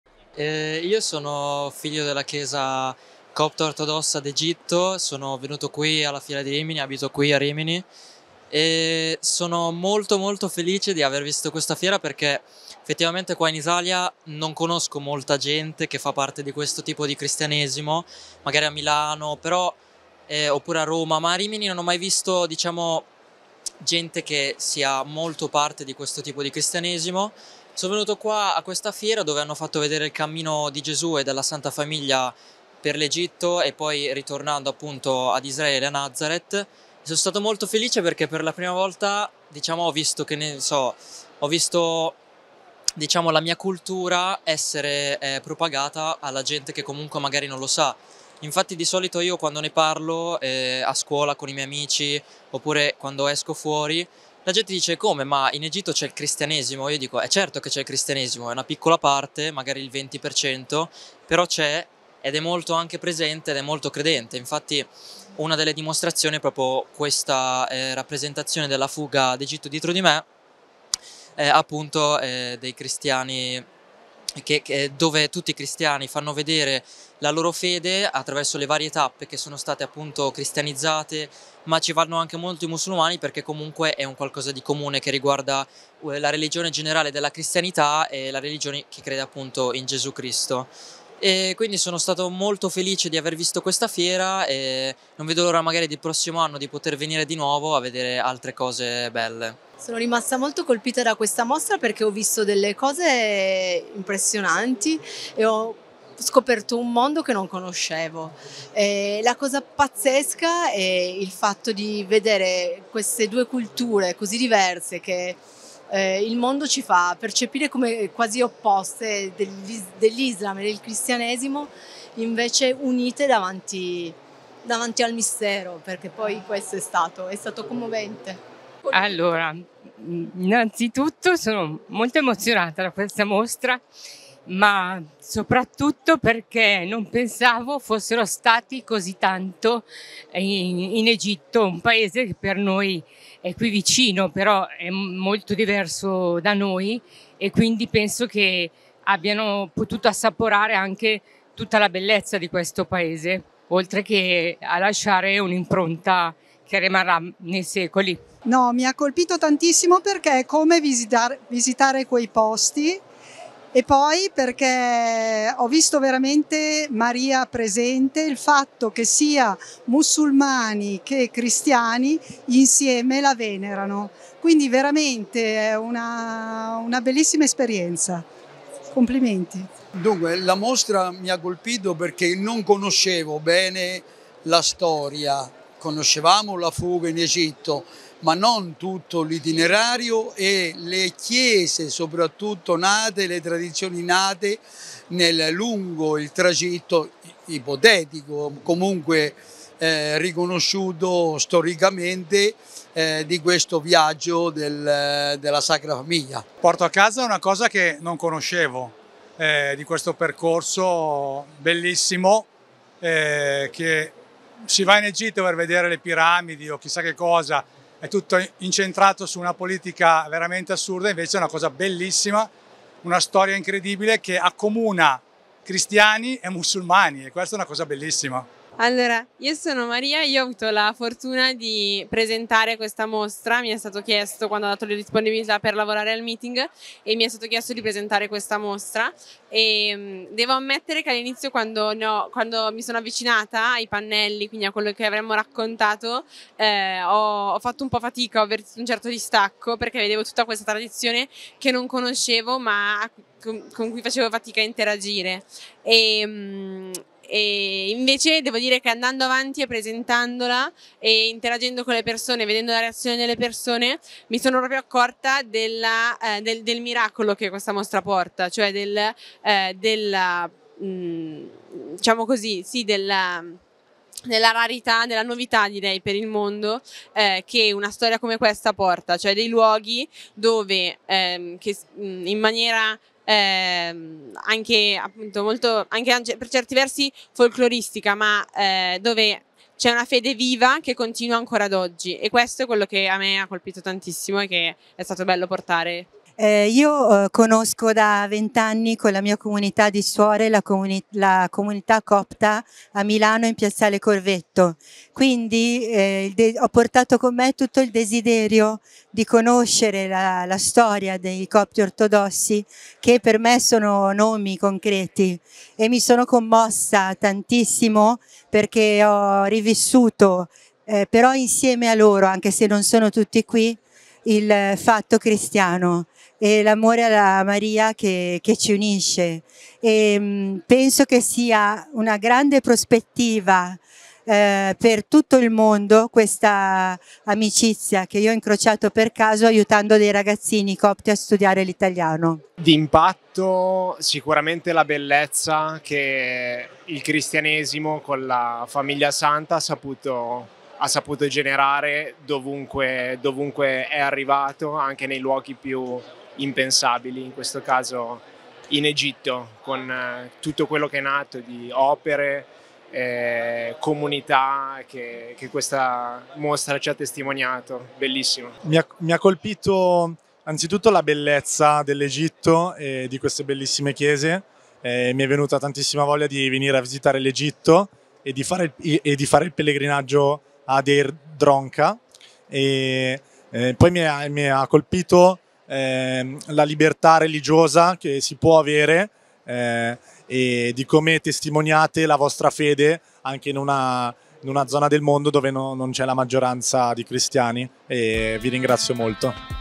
مسار رائع لرحلة أقرها التاريخ!! شاهد إعجاب الزائرين بمعرض ريميني الدولي بإيطاليا.
مسار رائع لرحلة أقرها التاريخ!!
 شاهد إعجاب الزائرين بمعرض ريميني الدولي بإيطاليا.